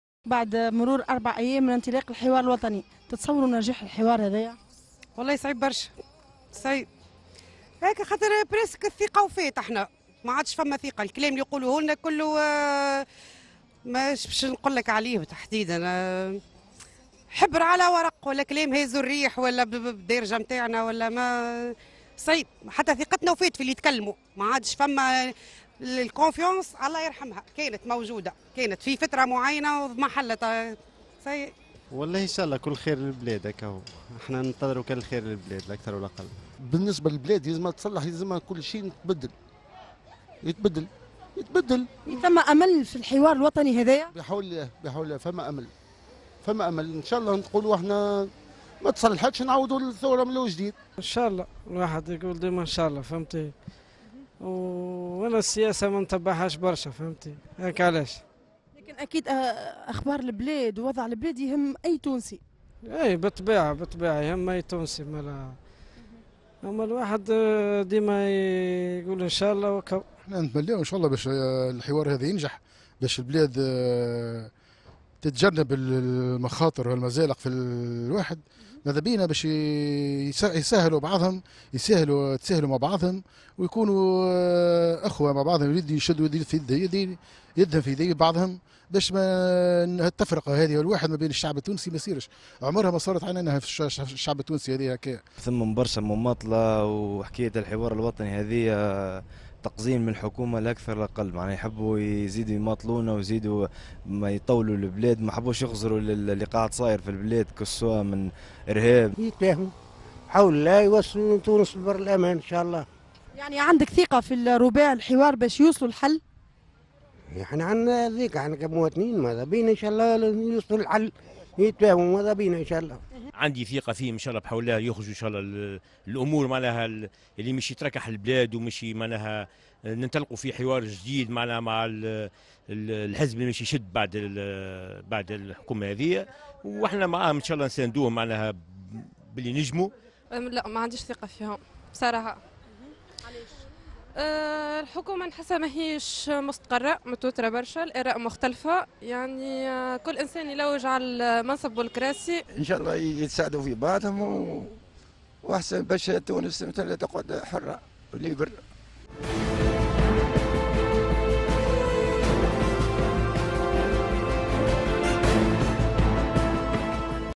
هل هناك أمل في نجاح الحوار الوطني ؟
آراء المواطنين